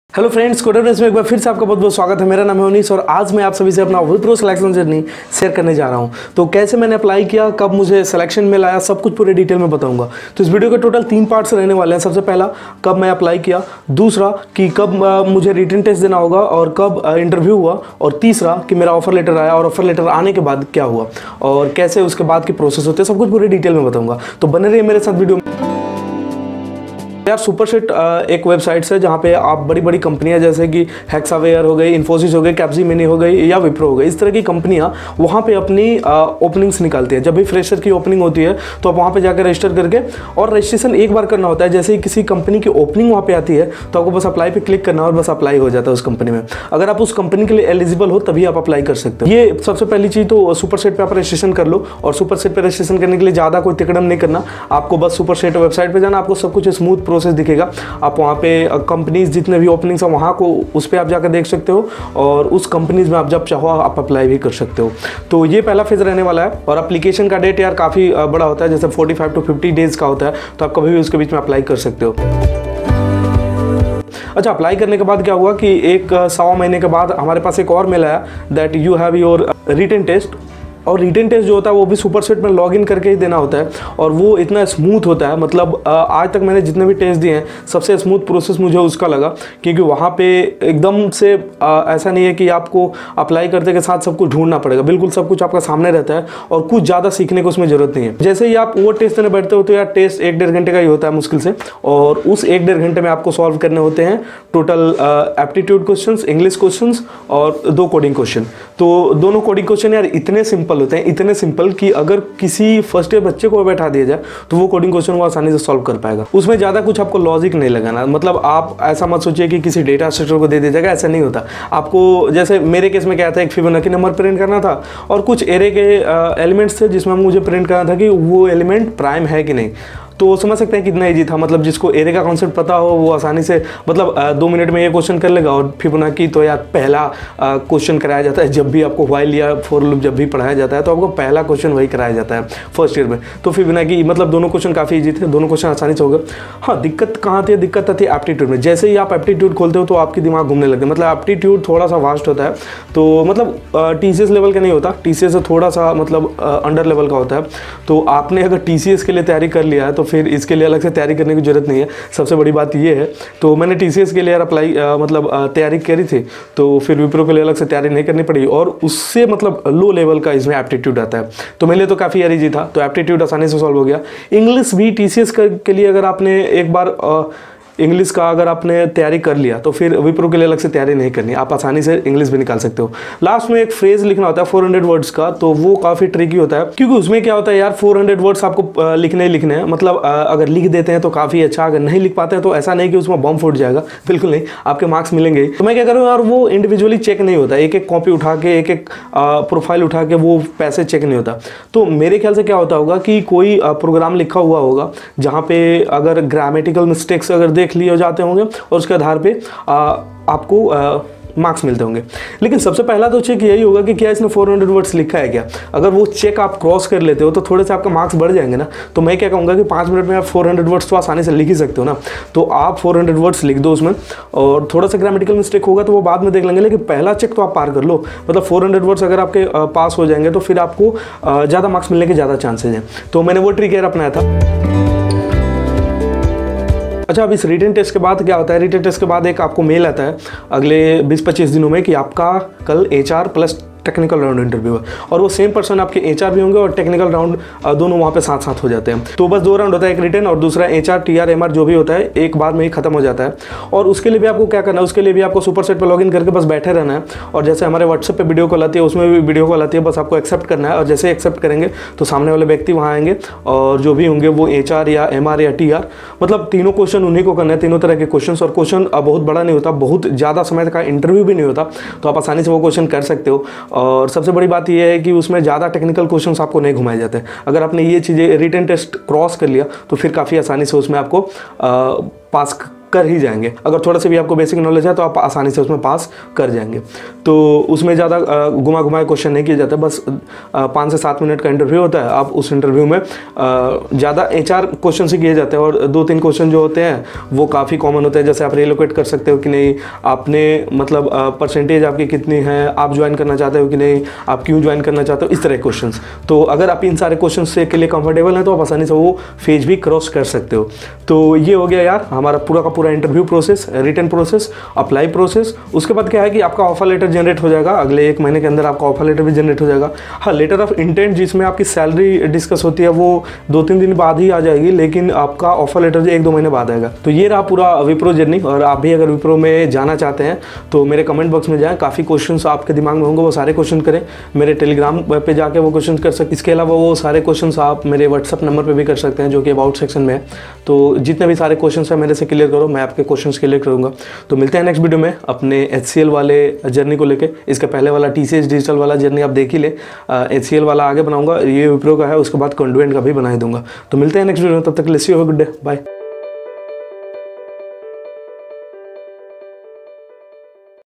My Wipro Selection Journey | How to get placements in Wipro | Questions asked in Wipro Interviews
In this particular video I have shared the wipro placements journey with you guys. I have shared all the phases in the video through which you can also grab placements in the wipro.